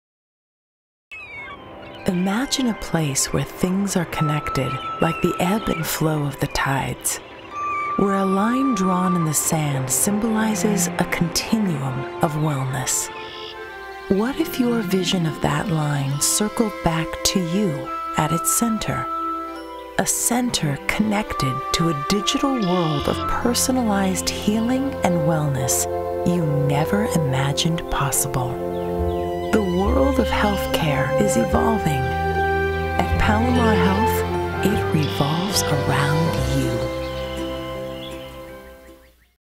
Palomar Health Commercial :30
:30 second broadcast spot featuring a new branding message for Palomar Health.